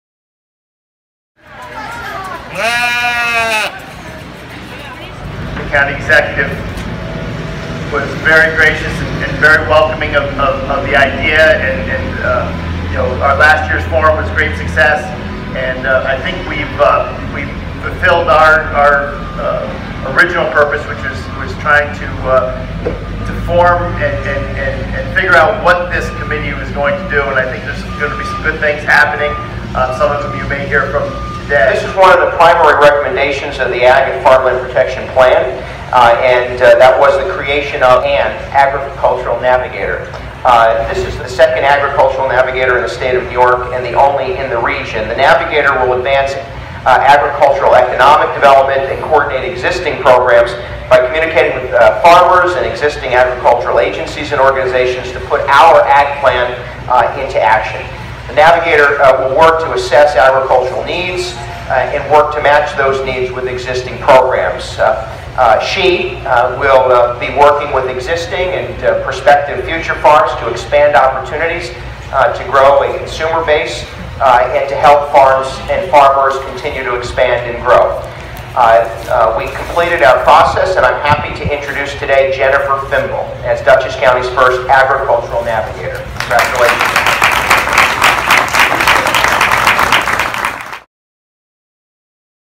County Executive Marc Molinaro Introduces Agricultural Navigator, Tours Livestock Barns
In addition to touring the Dutchess County Fair's livestock with other local leaders, Dutchess County Executive Marc Molinaro announced the appointment of Jennifer Fimbel as Dutchess County’s first Agricultural Navigator, a primary recommendation of the Agricultural and Farmland Protection Plan. Ms. Fimbel has more than 30 years of experience working in Agriculture and has a Masters of Agriculture from Oklahoma State University. As Agricultural Navigator through a contract with Cornell Cooperative Extension, she will be the front line person for farmers and agricultural agencies to turn to and will coordinate existing programs, inform and educate framers about new technologies, practices and regulations.  Dutchess County is the first county in the Hudson Valley region to appoint an Agricultural Navigator, and only the second county in New York State.